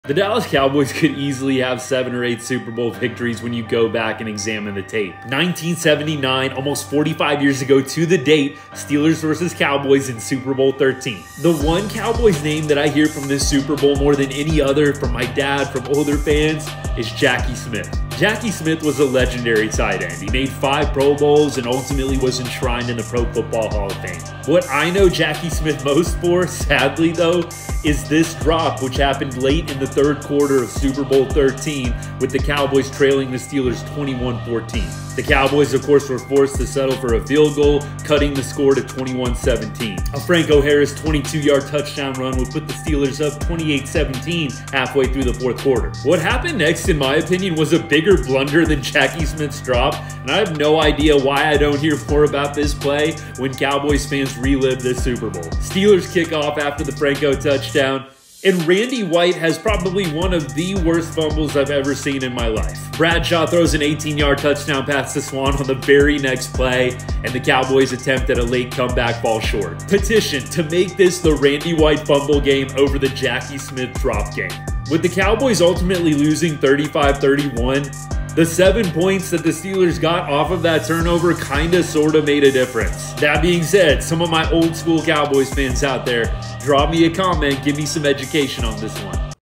Stop blaming Jackie Smith for the Cowboys’ loss in Super Bowl XIII
We have to stop blaming Jackie Smith for the Cowboys’ loss in Super Bowl XIII. Let’s take a little closer look at the tape of this classic matchup between the Cowboys and Steelers…